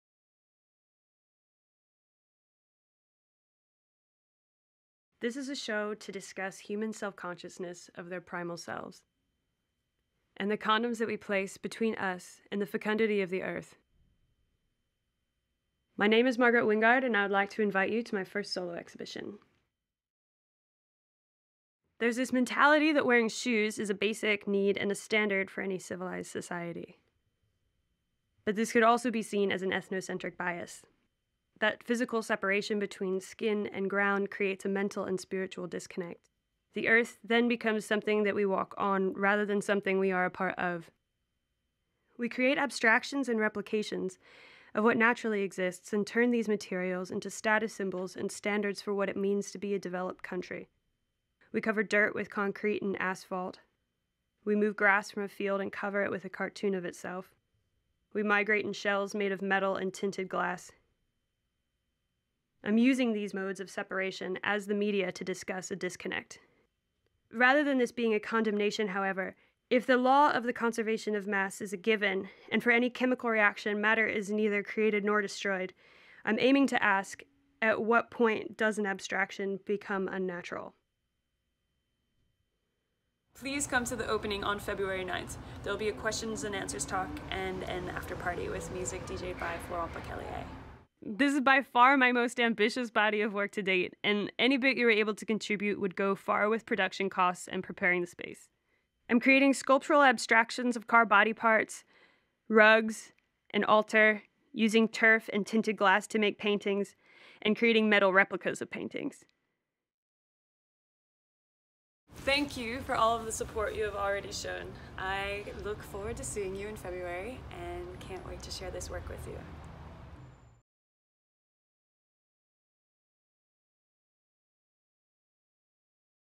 DELUXED - A solo exhibition by Margaret Wingard
An invitation and preview for a visual and conceptual art exhibition in Oakland, CA. February 2019

This show is about the collective self-consciousness humans have of their primal selves and the condoms that we place between us and the fecundity of the Earth.

There’s this mentality that wearing shoes is a basic human need and a standard for any ‘civilized society’; but, this could also be seen as an ethnocentric bias.

This physical separation between skin and ground creates a mental and spiritual disconnect. The Earth then becomes something that we walk on rather than something of which we are apart.

We create abstractions and replications of what naturally exists and turn these materials into status symbols and standards for what it means to be a developed country.

We cover dirt with concrete and asphalt.
We remove grass from a field and cover it with a cartoon of itself.
We migrate in shells made of metal and tinted glass.

These modes of separation are used as the media to discuss the disconnect.

Given the Law of the Conservation of Mass, however; and in any chemical reaction matter is neither created nor destroyed; the show aims to ask: At what point does an abstraction become unnatural?

Editor: Jeshua Bratman
Videographer: Josh Soyombo
Artistic Director: Margaret Wingard